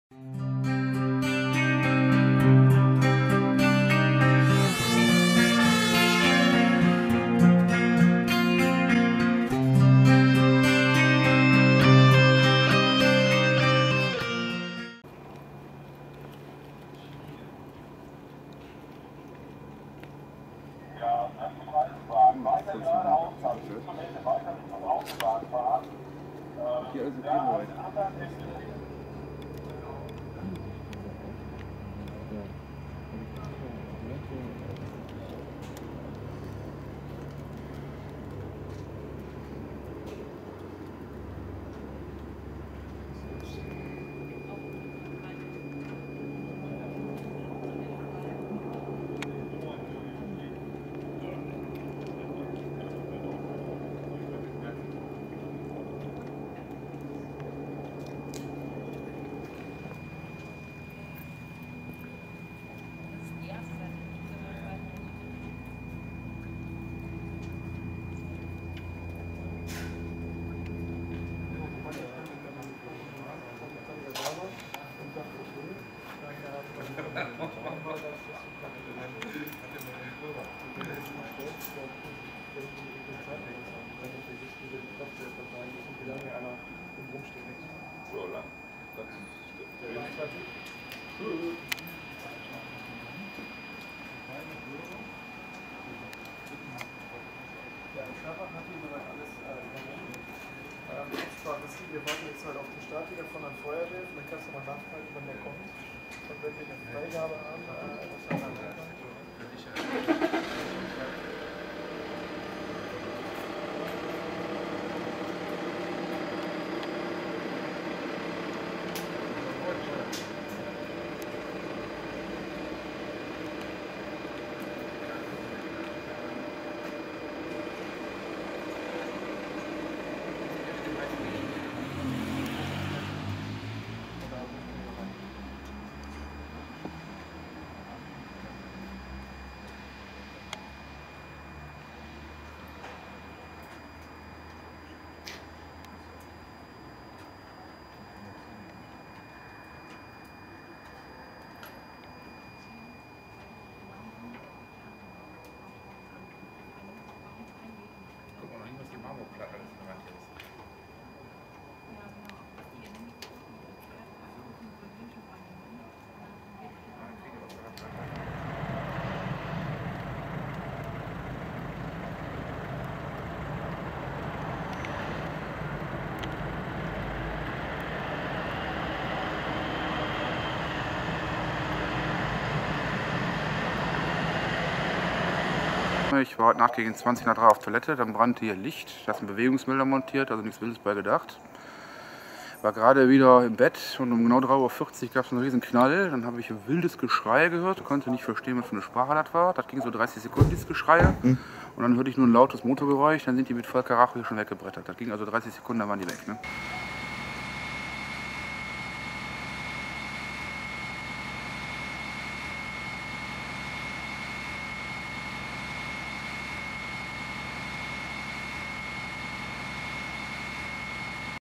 [GELDAUTOMAT IN MEERBUSCH GESPRENGT] ~ Commerzbank verwüstet || Täter flüchtig ~
Meerbusch-Osterath (ots) – In der Nacht zu Donnerstag (5.11.), gegen 3:35 Uhr, leiteten bislang unbekannte Täter einen aktuell nicht näher bestimmbaren Stoff in den Geldautomaten einer Bank an der Strümper Straße ein. Anschließend brachten sie das Selbstbedienungsgerät zur Detonation und gelangten an das darin befindlichen Bargeld. Über die Höhe des erbeuteten Betrages kann derzeit noch keine Aussage getroffen werden. Am Gebäude entstand erheblicher Sachschaden. Zeugen sahen nach der Tat drei maskierte Personen in einem dunklen Auto der Marke Audi flüchten.

Die Fahndung der Polizei, bei der auch ein Hubschrauber eingesetzt wurde, verlief bislang ohne Erfolg. Aktuell sichern Kriminalbeamte noch Spuren am Tatort. Ein Zusammenhang mit den zurückliegenden Sprengungen von Geldautomaten in Neuss aber auch in anderen Städten wird geprüft.

Die Polizei bittet mögliche weitere Zeugen, die Angaben zur Tat machen oder Hinweise zu dem Fluchtfahrzeug beziehungsweise zu dessen Insassen geben können, sich unter der Telefonnummer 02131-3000 mit den zuständigen Sachbearbeitern der Kripo in Verbindung zu setzen. Die Ermittlungen dauern an.

Textquelle: Pressemeldung Kreispolizeibehörde Neuss

Copyright : Emergency-Report.de